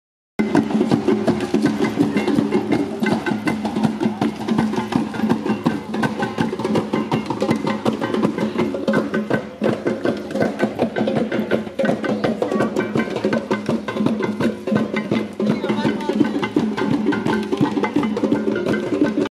danse autour du feu au pavillon africain à Auroville, Inde
wow c'était tellement transcendant et intense *___*